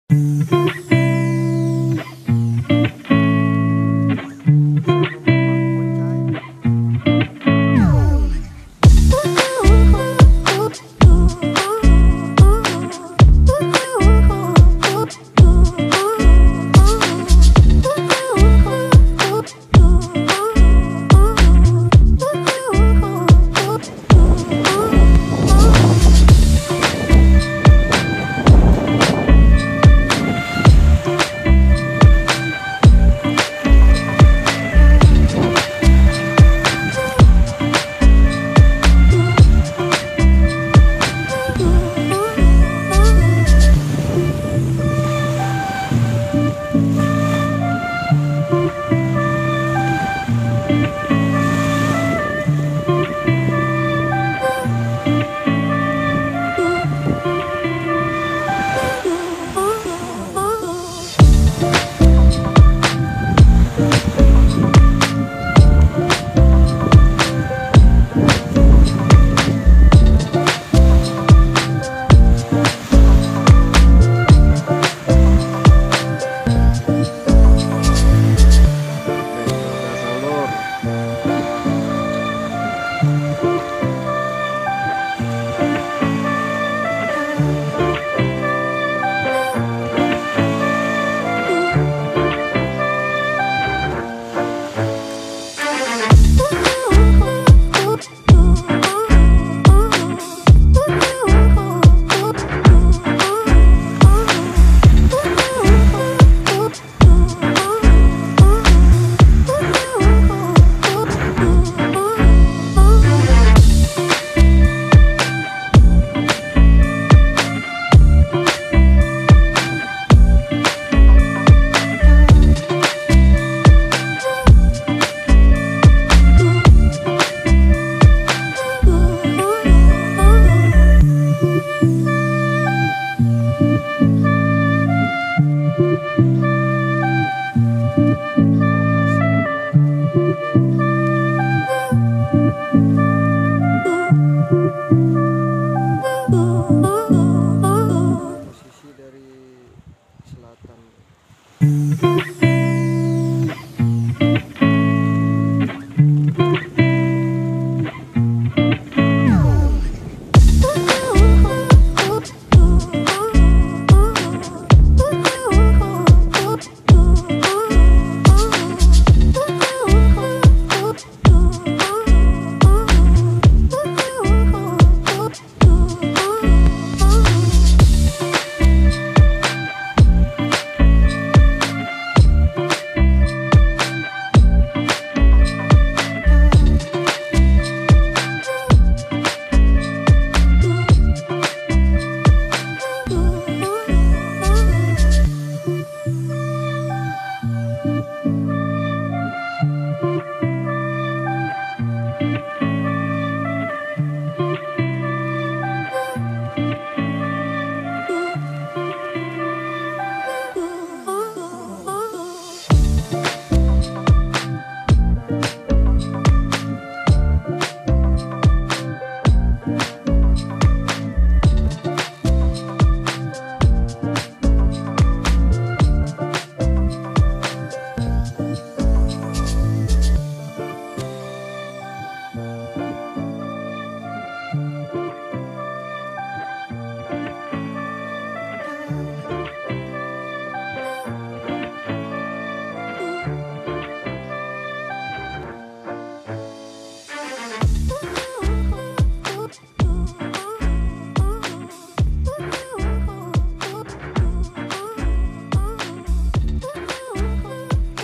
Kebanyakan Nelayan Libur melaut disaat cuaca masih kurang mendukung
cuaca buruk